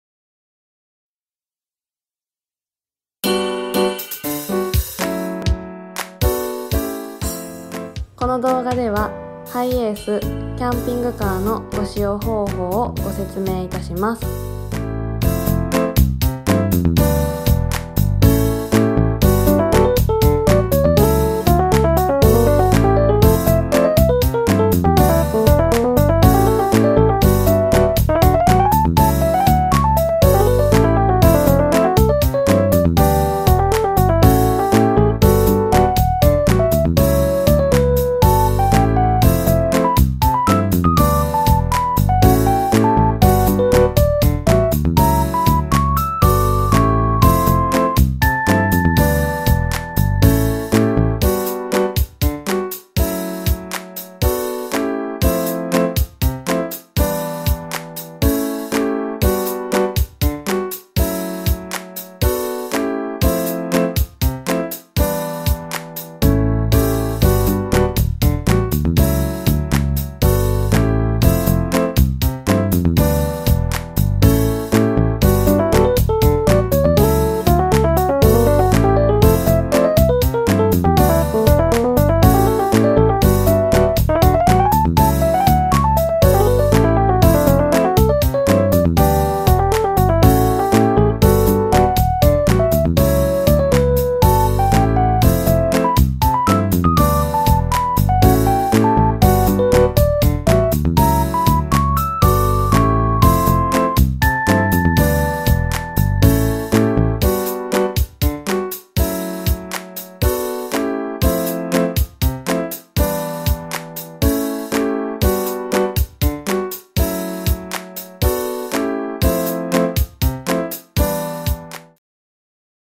【キャンピングカーレンタル】ハイエース・キャンピングカーの取り扱い説明
HondaCars三重のレンタカーサービス「HCMレンタカー」より、
ハイエース・キャンピングカーのご使用方法をご説明しております。

レンタカーについて詳しくはHCMレンタカーHPをご確認ください。